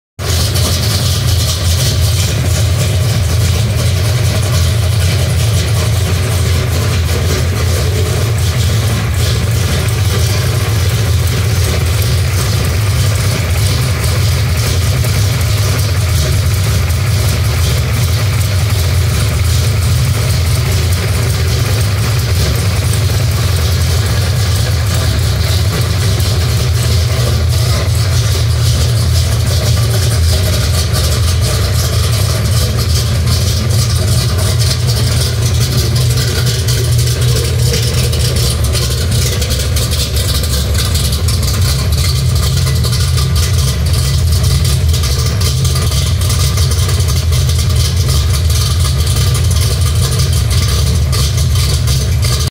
1969 Mustang Mach1 Holley EFI
Cold start up on Mustang Mach1 with Holley Terminator Stealth.